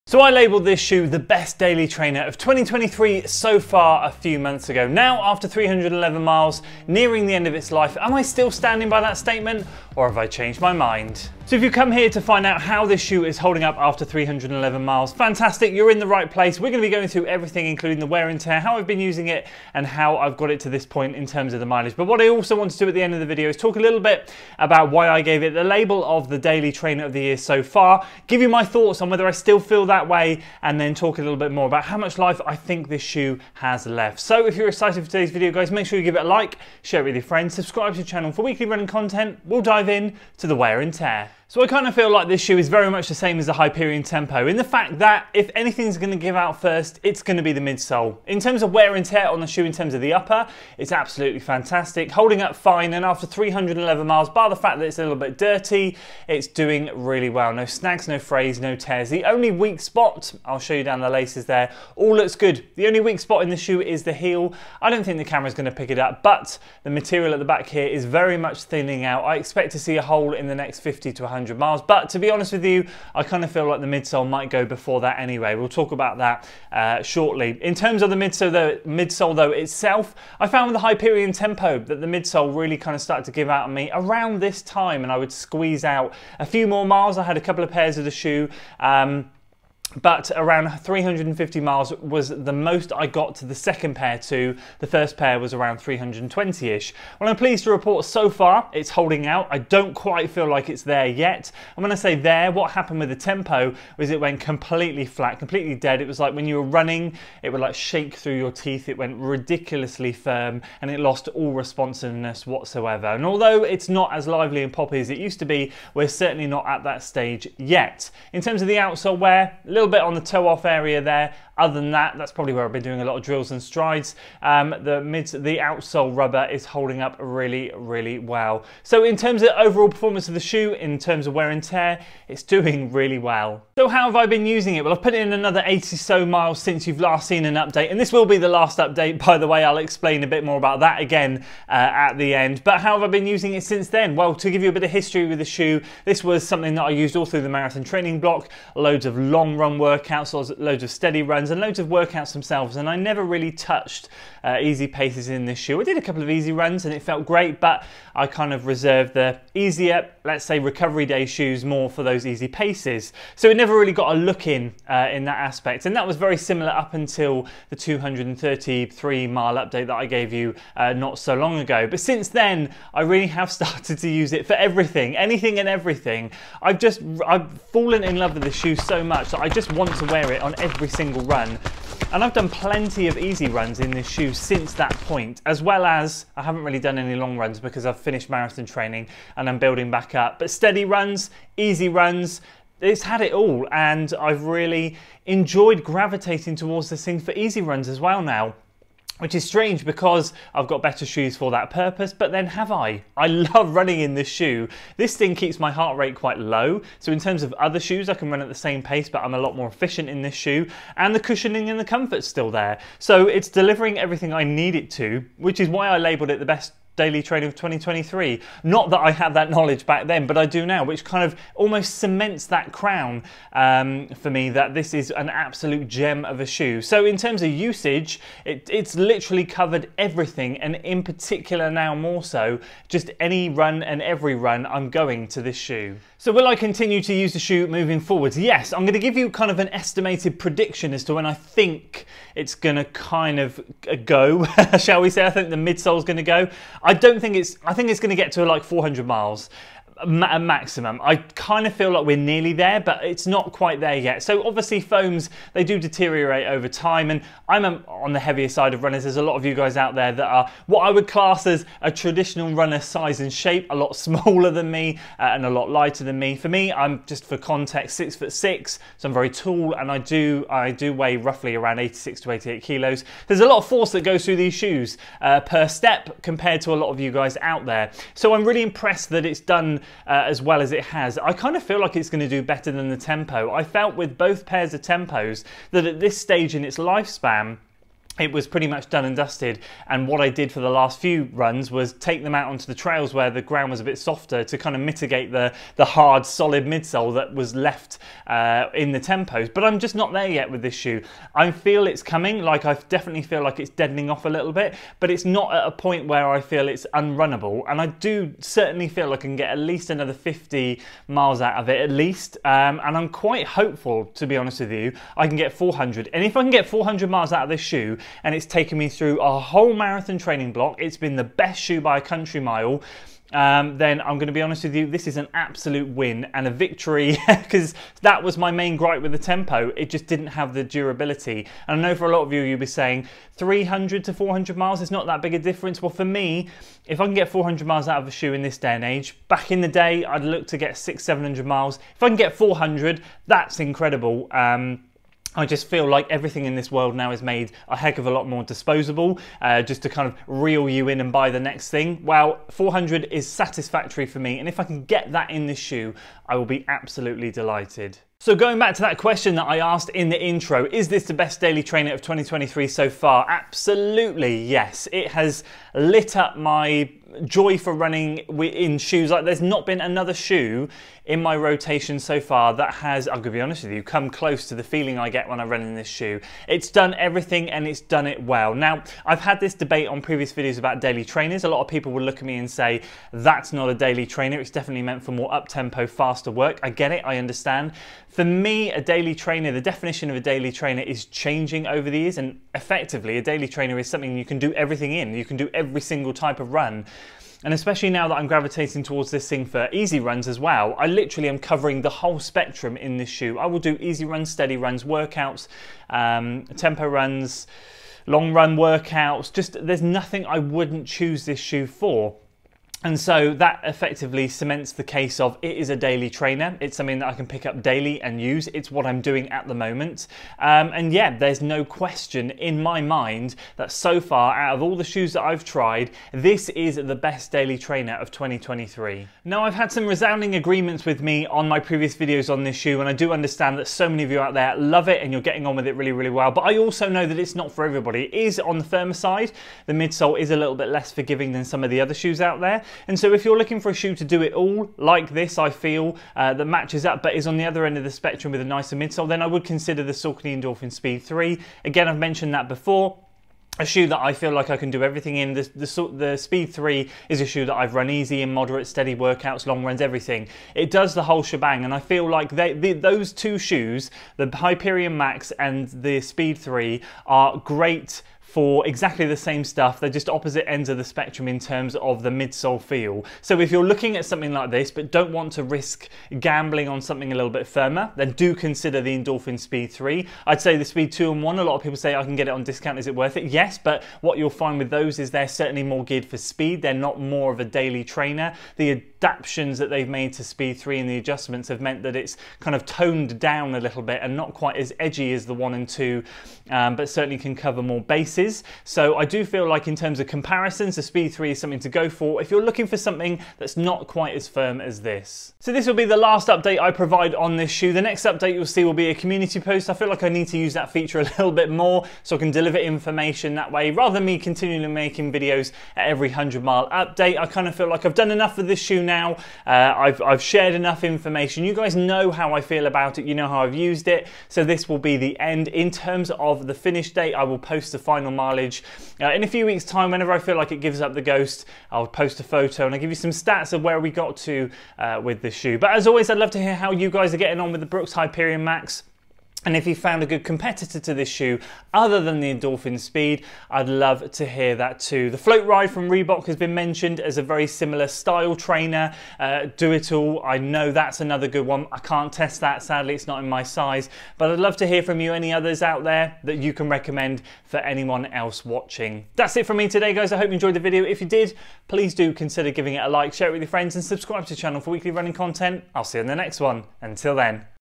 Is This Still The Best Daily Trainer Of 2023 So Far??
Is This Still The Best Daily Trainer Of 2023 So Far?? welcome back to another video and today I am sharing with you an update on the brooks hyperion max running shoe after 311 miles and answering the question "is this still the best daily trainer of 2023 so far??".  This shoe has done it all for me but its getting close to the end of its life now... how many more miles can I get out of it??

How are you getting on with your brooks hyperion max?